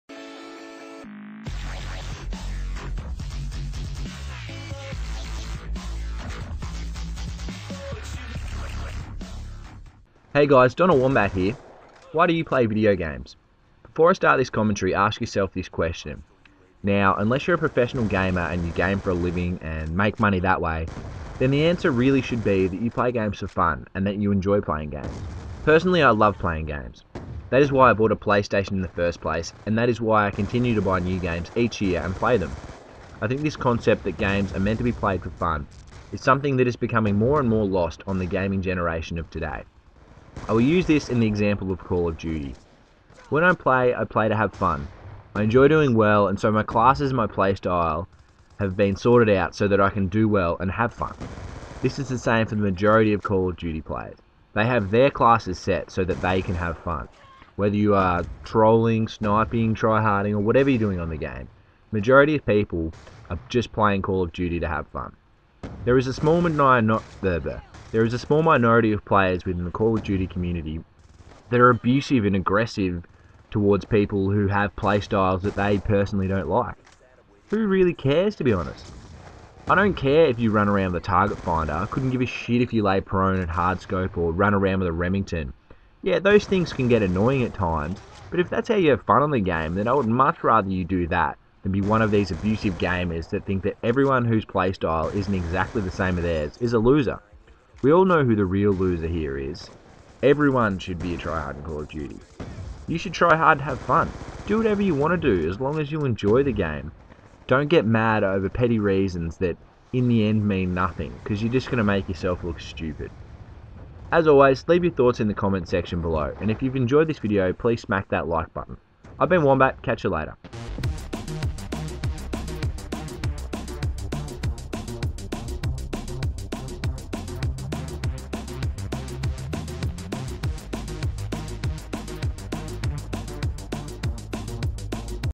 Swarm With Every Gun - Scorpion - Play Games to Have Fun
In this video I talk about why you should play games and to make sure you don't get mixed up. Hope you enjoy. Wombat